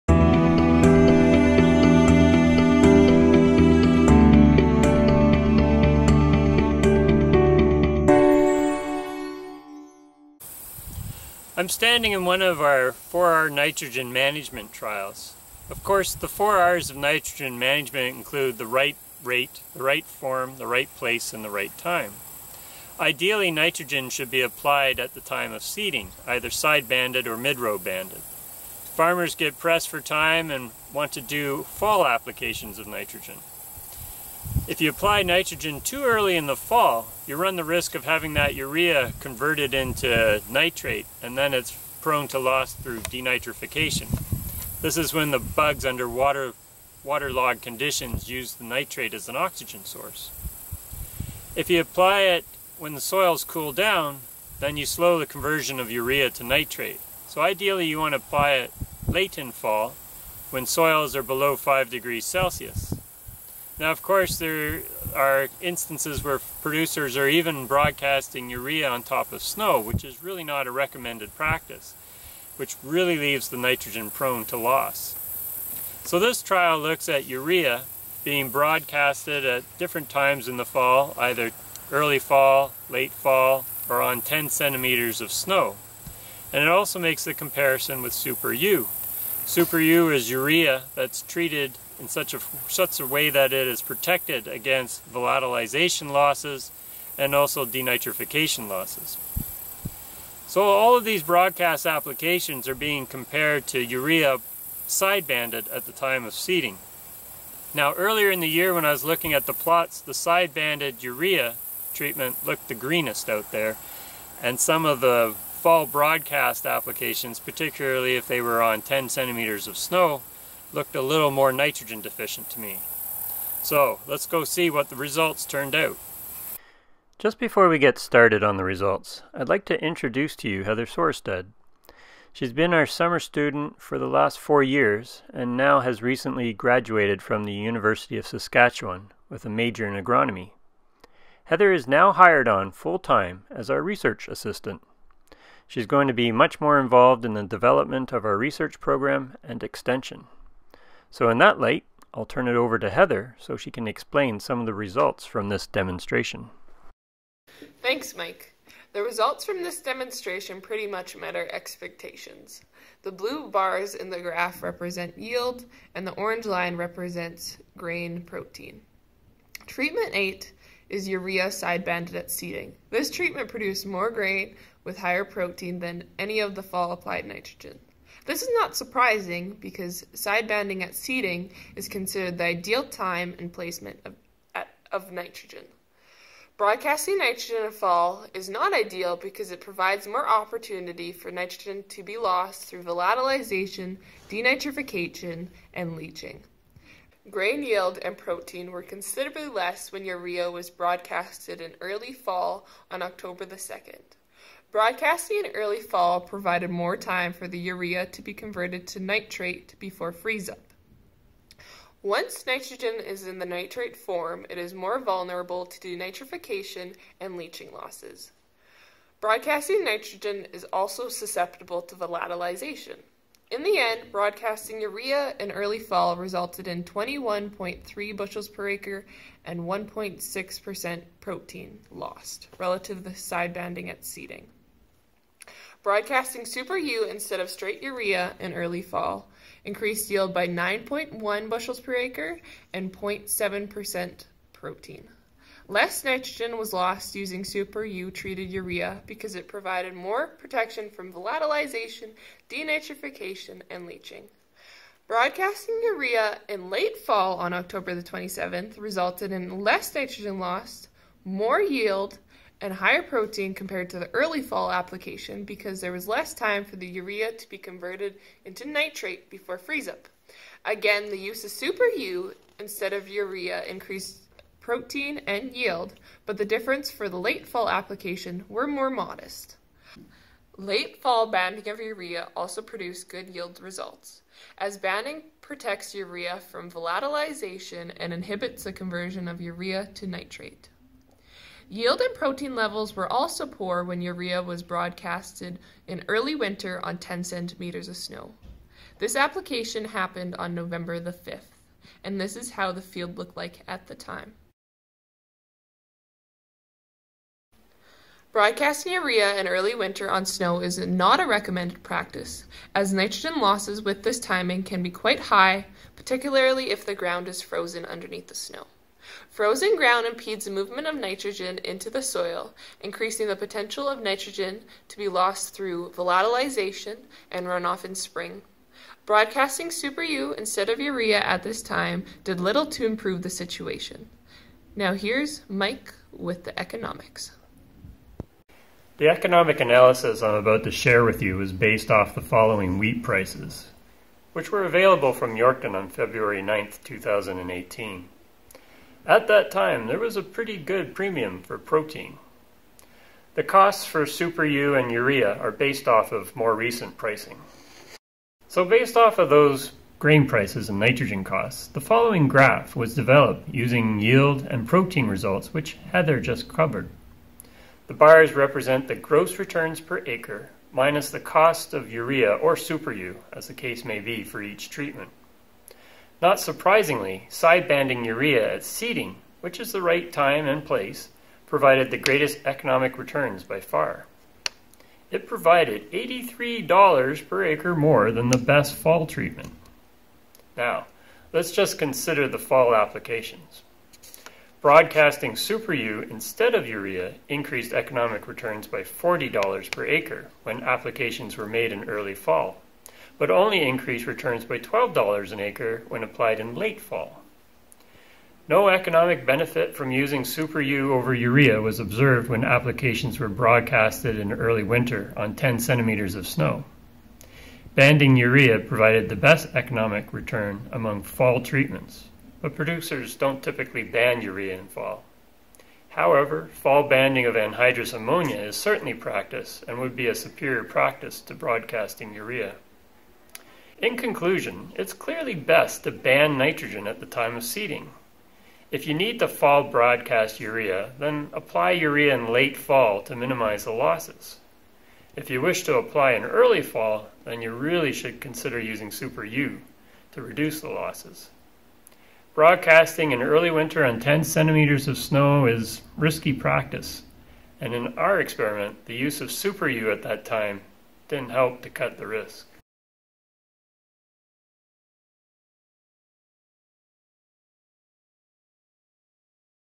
4R Fall Applied Urea to Spring Wheat 2018
Researcher's Michael Hall & Heather Sorestad speak on the results of the 2018 trial showing the effect of applied Urea and SUPERU to spring wheat via the 4R methodology.